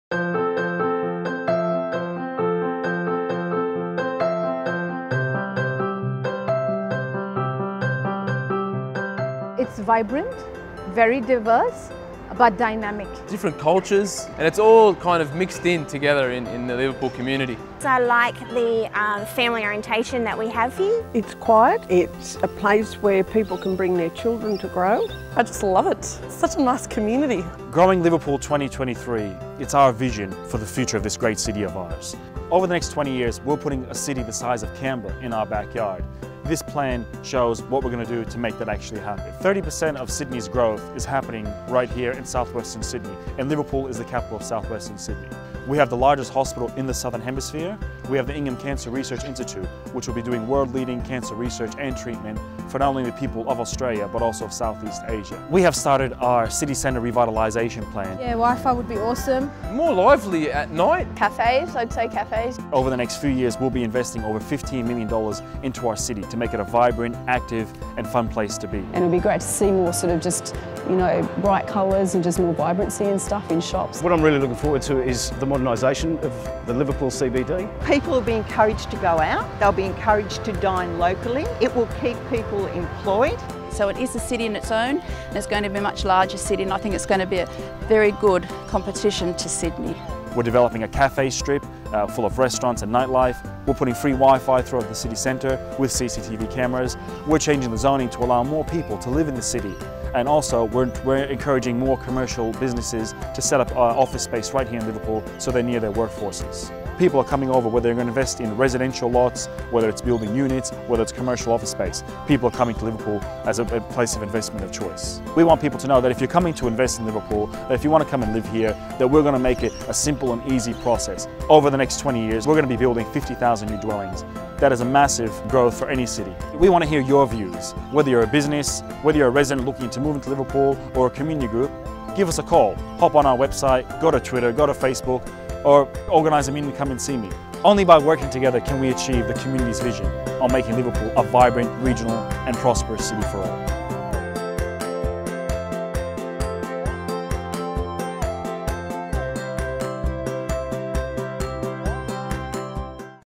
Growing Liverpool 2023 Video 15 April 2014
GROWING LIVERPOOL 2023

The plan identifies seven strategic directions which have been developed following an in-depth and ongoing community consultation process. Each direction includes a number of strategies that need to be delivered in order to achieve the community's vision for a vibrant regional city of opportunity, prosperity and diversity. Growing Liverpool 2023 was adopted by Council at its meeting on 26 June 2013.

The Growing Liverpool 2023 Delivery Program and Operational Plan outline how Council will deliver on the strategies identified in the Community Strategic Plan.